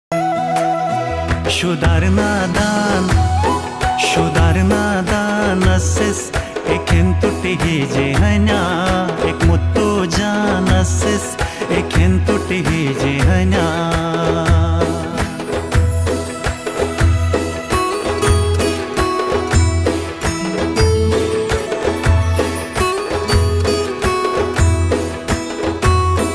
Polo 🐎 Traditional game of Gilgit Baltistan|New shina song|Hunza valley|Culture of Gilgit Baltistan
Polo is the traditional game of Gilgit Baltistan culture of GB. Boy playing it with shina song in the background